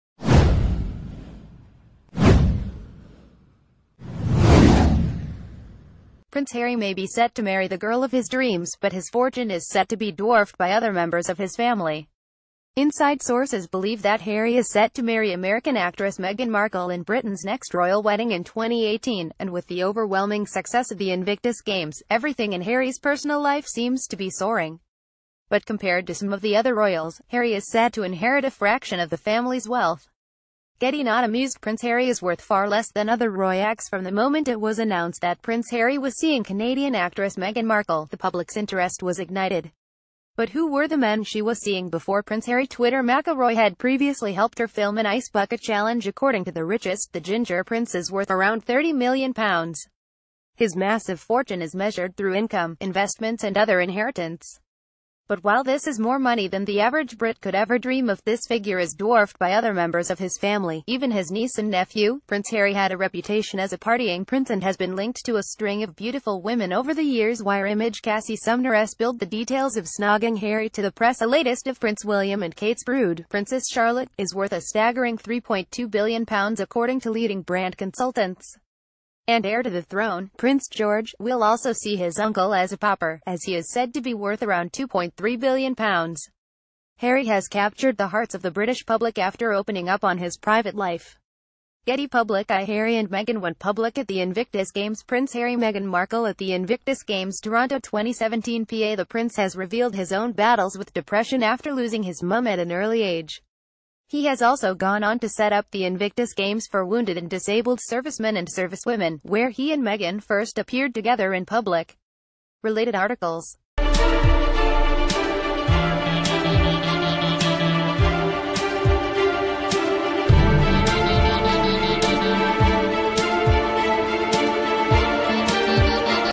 Breaking News | Prince harry’s fortune revealed – and it’s not what you’d think
Prince harry’s fortune revealed – and it’s not what you’d think

PRINCE Harry may be set to marry the girl of his dreams, but his fortune is set to be dwarfed by oth...